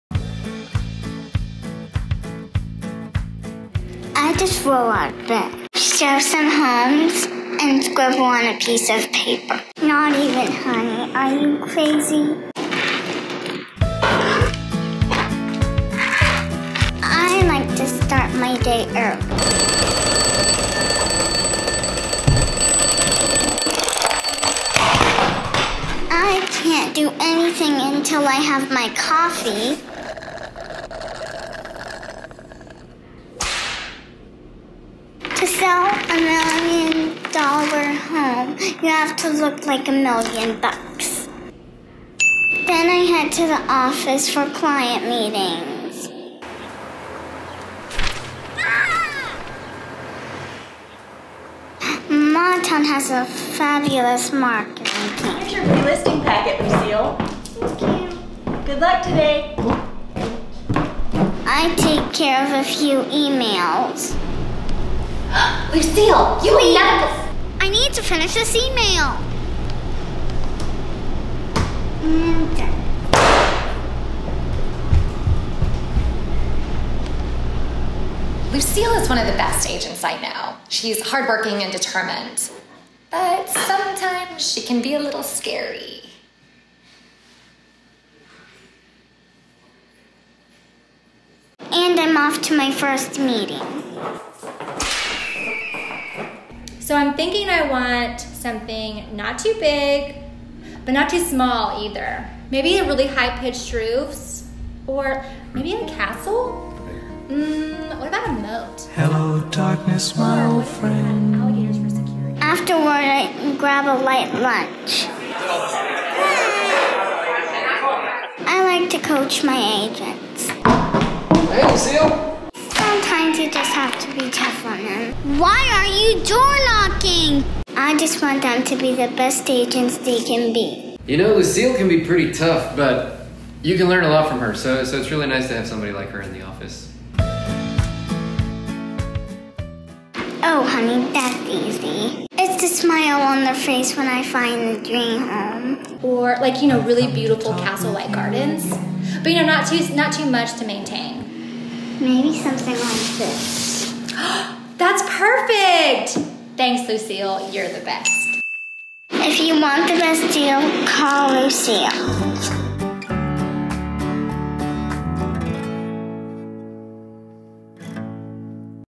Lucille, the Realtor Kid — ABC's of Real Estate
Meet Modtown's newest agent, Lucille. When she's not closing deals, she's crushing this adulting thing.